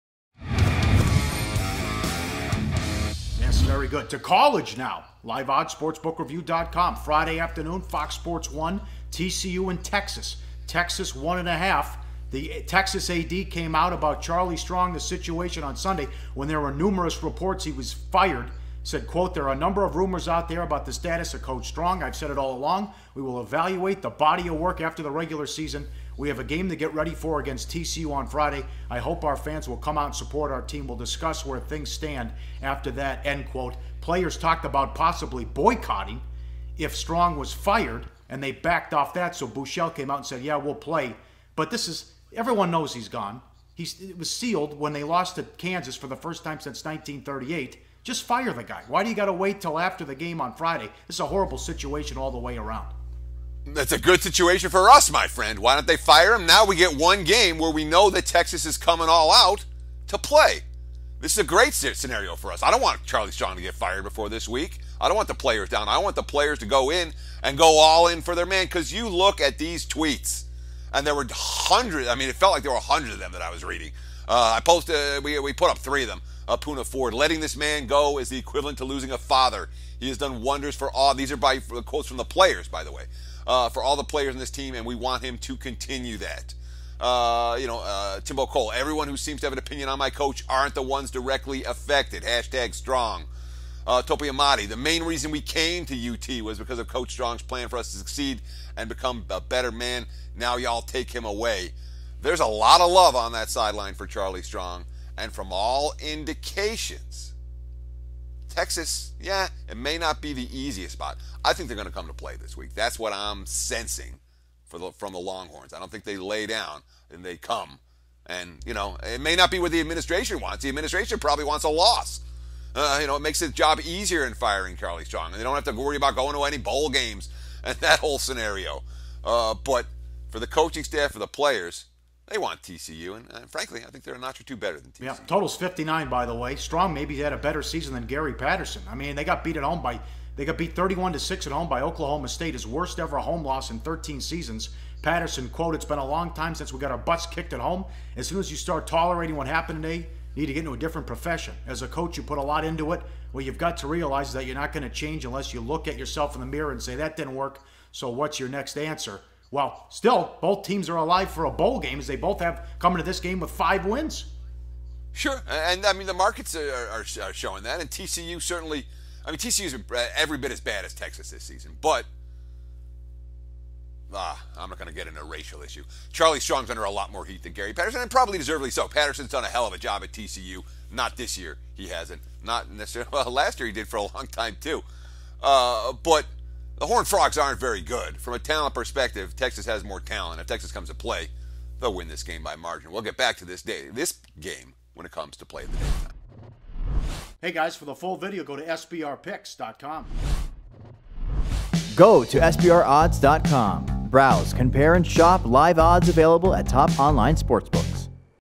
TCU vs. Texas Longhorns College Football Picks | Sports BIT | NCAAF Rivalry Week
It's College Football's Rivalry Week! The guys from Las Vegas break down the TCU Horned Frogs vs. Texas Longhorns for you like it was Sports Center for Sports Bettors! Compare over 200 NCAAF betting sites

Automated Video Transcription:

that's very good to college now live odds
sportsbook review dot-com friday
afternoon foxsports1 TCU in Texas Texas
one-and-a-half the Texas ad came out
about Charlie Strong the situation on
Sunday when there are numerous reports
he was fired said quote there are a
number of rumors out there about the
status of code strong I've said it all
along we will evaluate the body of work
after the regular season we have a game
to get ready for against tcu on friday i
hope our fans will come out support our
team will discuss where things stand
after that end quote players talked
about possibly boycotting if strong was
fired and they backed off that so blue
shell came out so yeah we'll play but
this is everyone knows he's gone he's it
was sealed when they lost the Kansas for
the first time since nineteen
thirty-eight just fire the guy
why do you gotta wait till after the
game on Friday it's a horrible situation
all the way around
that's a good situation for us my friend
why did they fire him now we get one
game where we know that Texas is coming
all out to play this is a great scenario
for us i don't want Charlie trying to
get fired before this week I don't want
the players down i want the players to
go in and go all in for their man
because you look at these tweets and
there were hundreds I mean it felt like
a hundred that i was reading i posted
where we put up three them up on the
four letting this man go is the
equivalent to losing a father he's done
wonders for all these are by for a quote
from the players by the way for all the
players on this team and we want him to
continue that you know a typical
everyone who seems to have an opinion on
my coach aren't the ones directly
affected hashtag strong topical body the
main reason we came to UT was because
the coach strong plan for us to succeed
and become a better man now y'all take
him away
there's a lot of love on that sideline
for Charlie Strong and from all
indications
texas yeah it may not be the easiest
spot I think they're gonna come to play
this week that's what I'm sensing for
the from the Longhorns I don't think
they lay down and they come and you know
it may not be with the administration
wants the administration probably wants
a loss